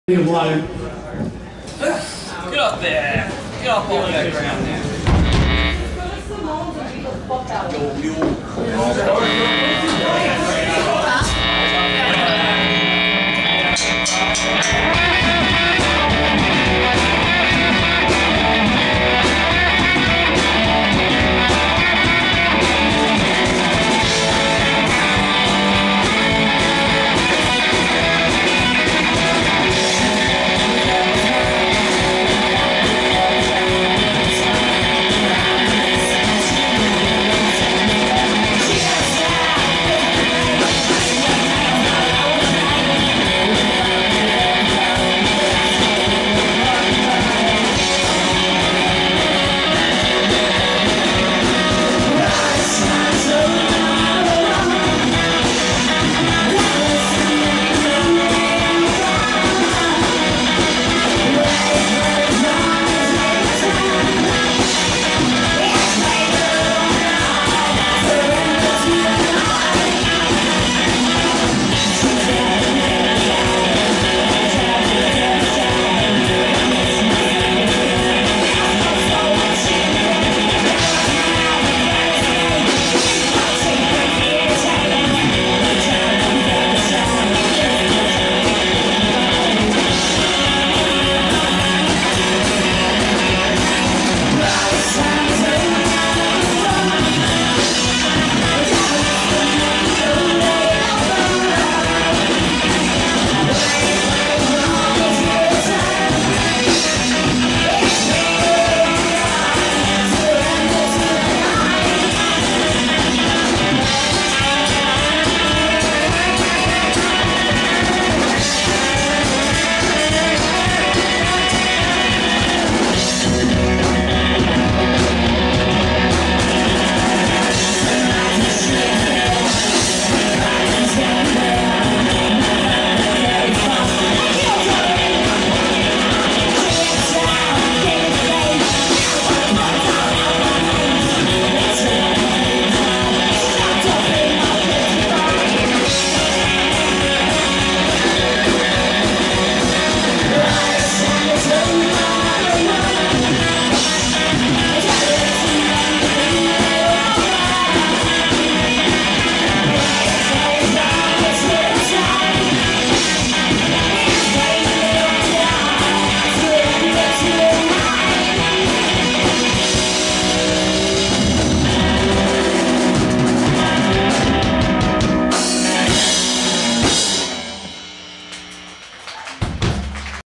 Super Sonic Space Rebels CHOPPED 55 Feb 24 2019
at Reggies with Three Blue Teardrops and Gutter Demons
Videography By AJE SUBSIST STUDIO